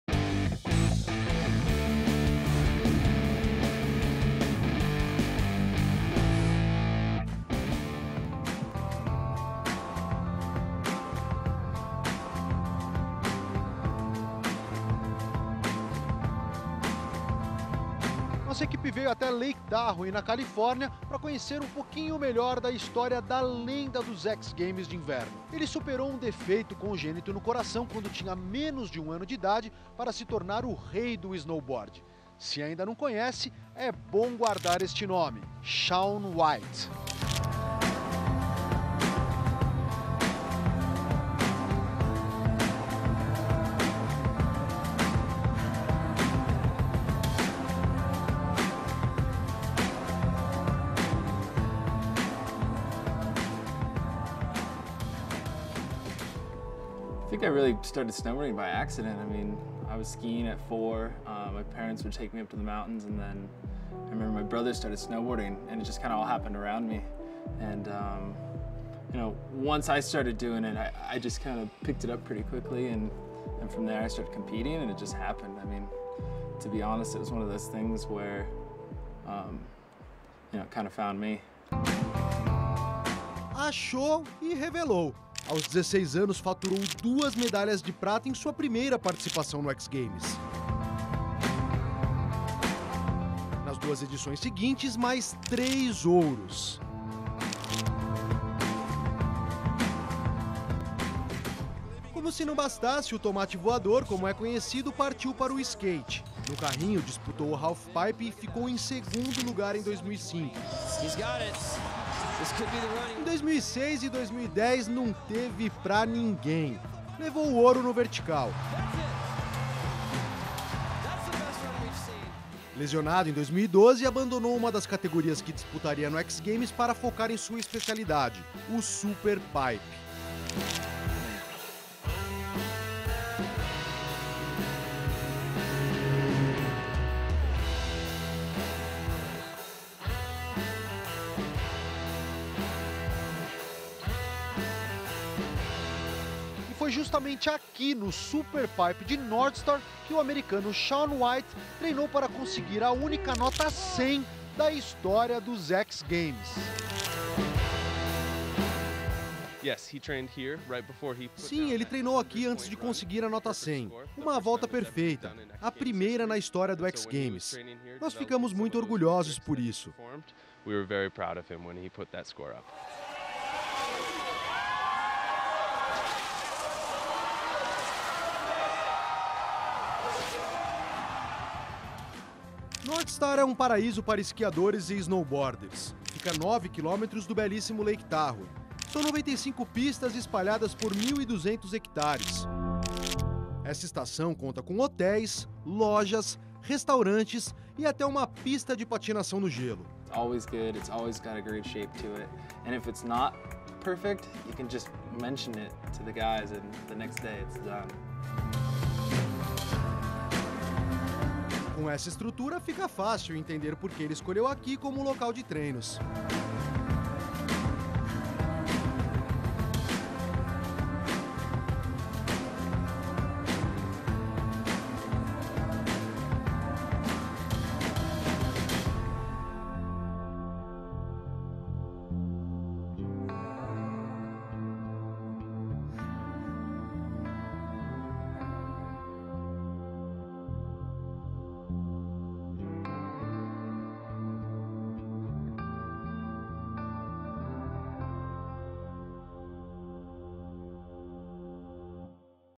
Shaun White Northstar com Fernando Navarro
Matéria veiculada no Redetv esporte de 08/01/13 sobre seus treinos no superpipe de Northstar na California.